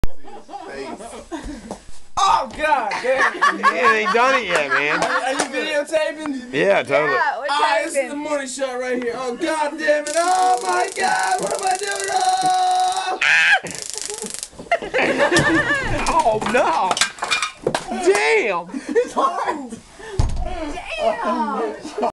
Stun Gun Meets Testicals... Lucky for you, it's dark.
Yeah, you can hear him as he involuntarily shot across the room and into my desk. A little blood, but alot more laughs!!!